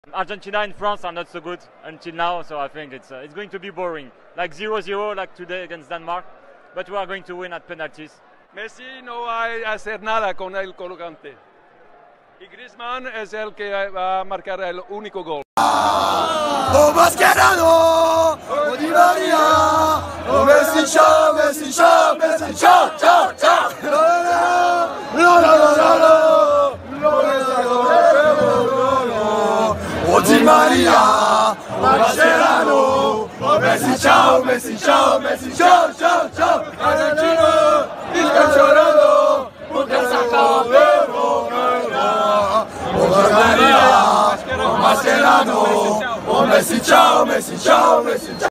🇫🇷Franceses opinan del partido contra 🇦🇷Argentina-*RS
Previo a choque del Sábado donde Argentina se medira con Francia por los 8vos de final del Mundial de Fútbol, estos hinchas franceses+ 1 Brazilero dan sus opiniones.
Al final se inventan un verso(La casa de papel)